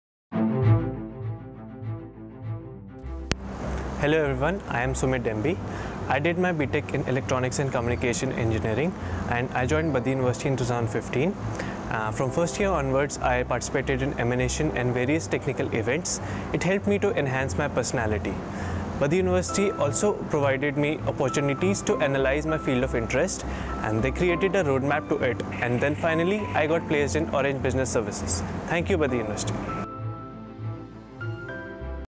Baddi University Students Placements Videos | Best Placement University
See the bigger picture
Students of Baddi University shares their views regarding placement opportunities at Baddi University. BUEST congratulates them and wishing good luck for bright future.